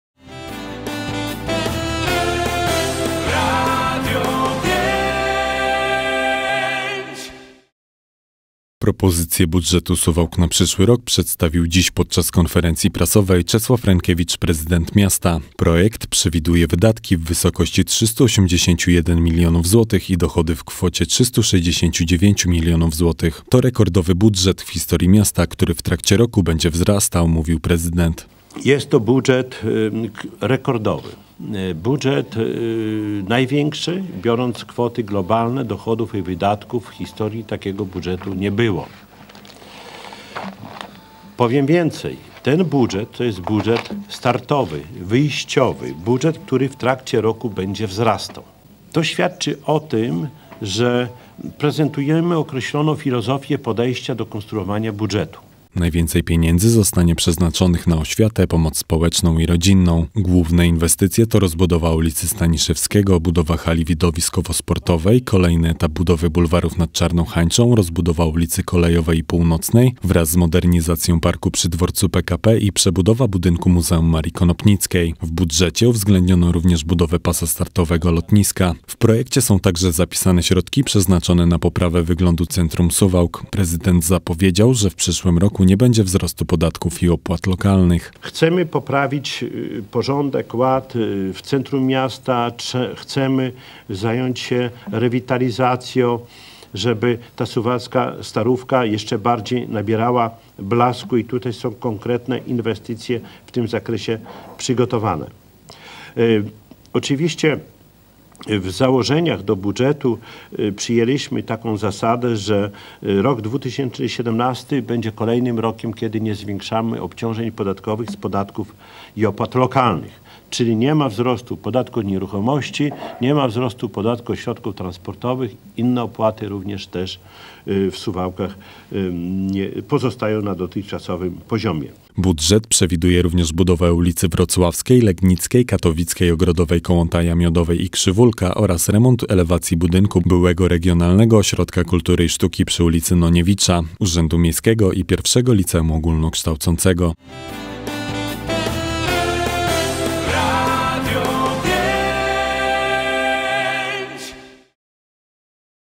Budżet na 2017 rok
Propozycję budżetu Suwałk na przyszły rok przedstawił dziś podczas konferencji prasowej Czesław Renkiewicz, prezydent miasta. Projekt przewiduje wydatki w wysokości 381 milionów złotych i dochody w kwocie 369 milionów złotych.
- To rekordowy budżet w historii miasta, który w trakcie roku będzie wzrastał- mówił prezydent
Najwięcej pieniędzy zostanie przeznaczonych na oświatę, pomoc społeczną i rodzinną.
Główne inwestycje to rozbudowa ulicy Staniszewskiego, budowa hali widowiskowo - sportowej, kolejny etap budowy bulwarów nad Czarną Hańczą, rozbudowa ulicy Kolejowej i Północnej wraz z modernizacją parku przy dworcu PKP i przebudowa budynku Muzeum Marii Konopnickiej.
W budżecie uwzględniono również budowę pasa startowego lotniska.
W projekcie są także zapisane środki przeznaczone na poprawę wyglądu centrum Suwałk. Prezydent zapowiedział, że w przyszłym roku nie będzie wzrostu podatków i opłat lokalnych
Budżet przewiduje również budowę ulicy Wrocławskiej, Legnickiej, Katowickiej, Ogrodowej, Kołłątaja, Miodowej i Krzywólka oraz remont elewacji budynku byłego Regionalnego Ośrodka Kultury i Sztuki przy ulicy Noniewicza, Urzędu Miejskiego i I Liceum Ogólnokształcącego.